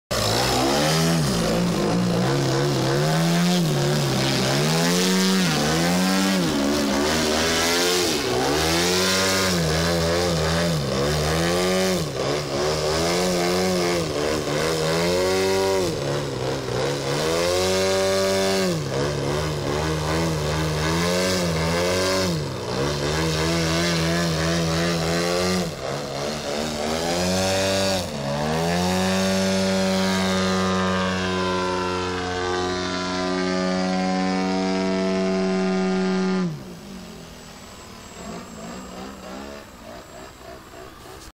yak 54 rc 2,7m,DLE 111, pilot Marek Matějka visení na vrtuli super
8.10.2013 bylo fakt krásně letiště velká dobrá Kladno